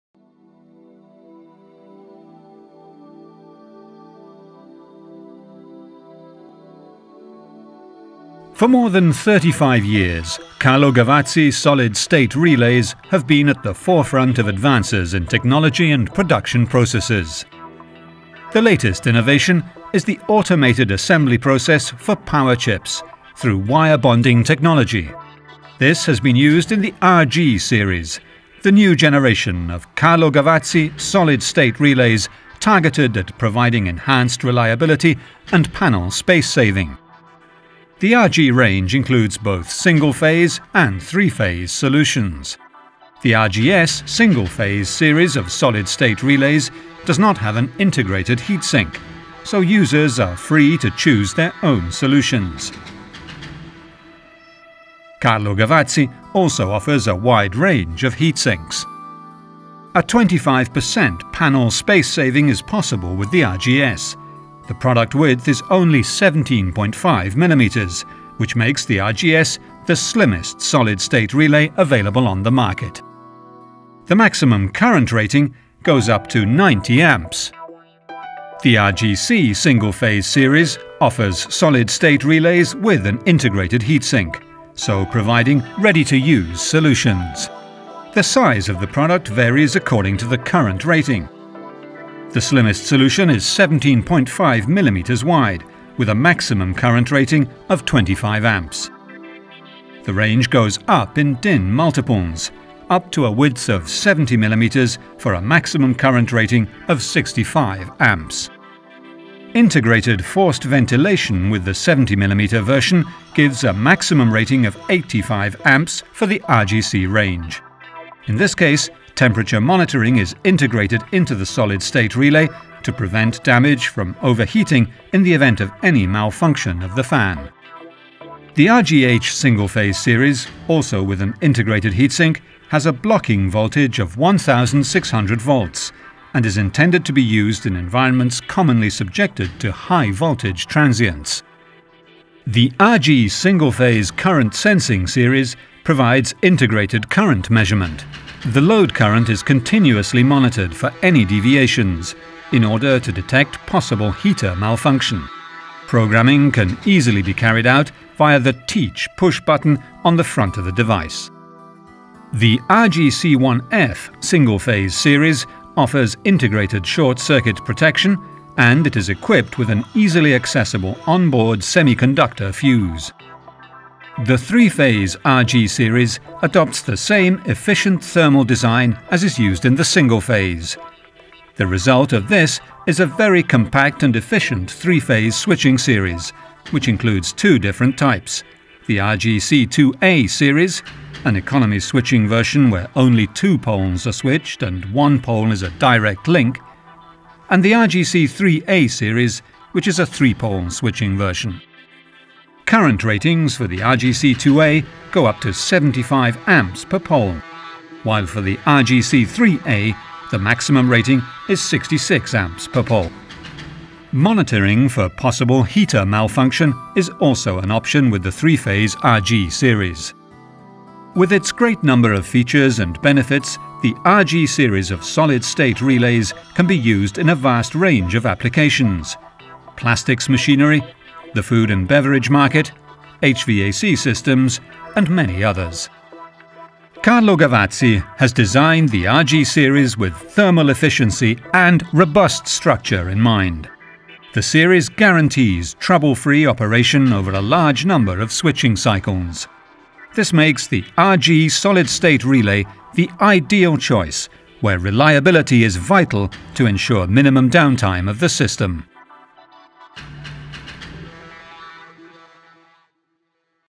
RG Series Solid State Relays from Carlo Gavazzi
An overview of Carlo Gavazzi's RG Series of single and three phase solid state relays with integrated heatsink.